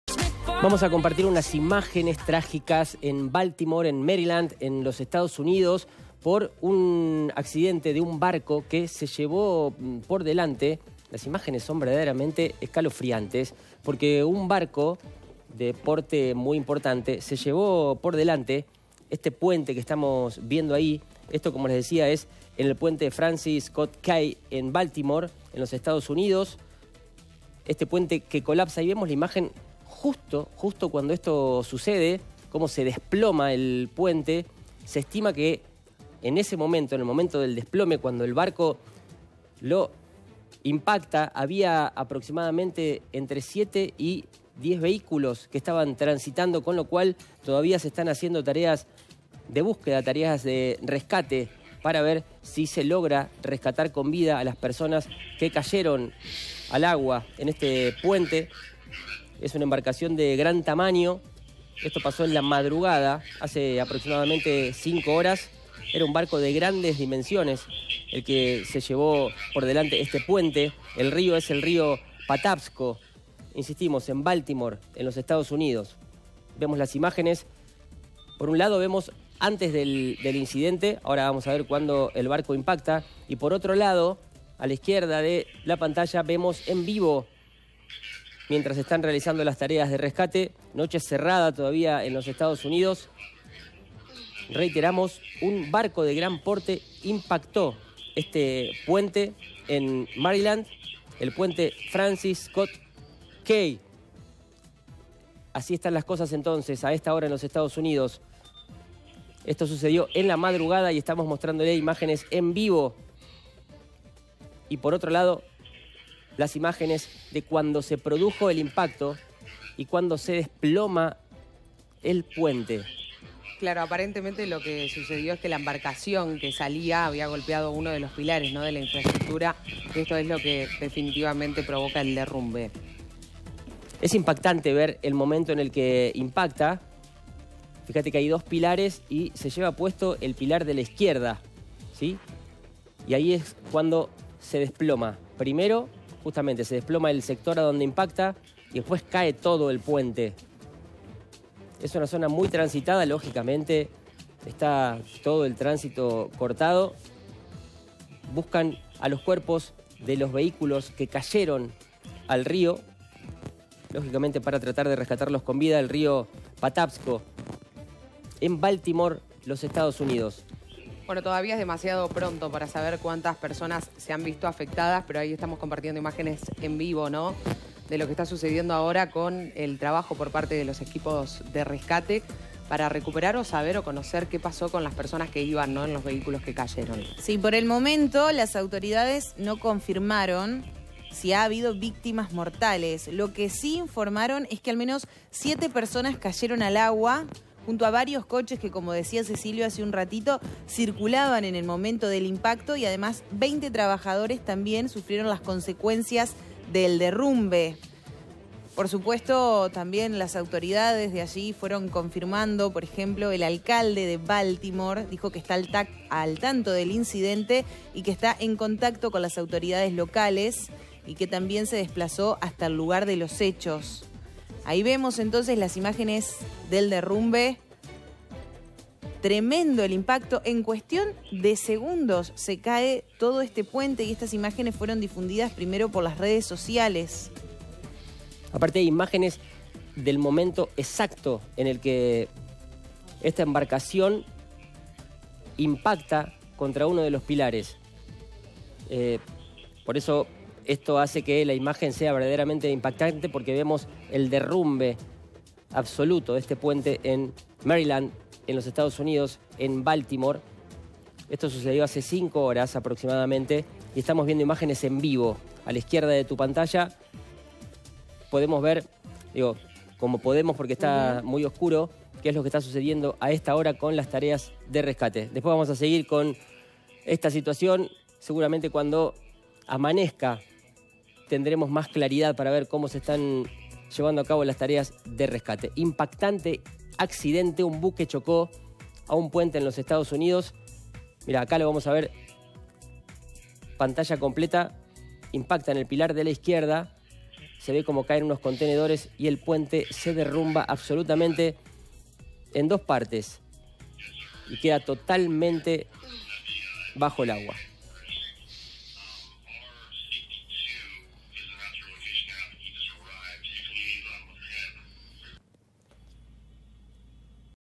EEUU - EN VIVO | Se derrumba un puente en Baltimore al ser chocado por un buque
👉 Un gran buque de carga chocó contra uno de los pilares del puente Francis Scott Key de Baltimore (costa este de EEUU) y produjo su derrumbe en la madrugada de este martes.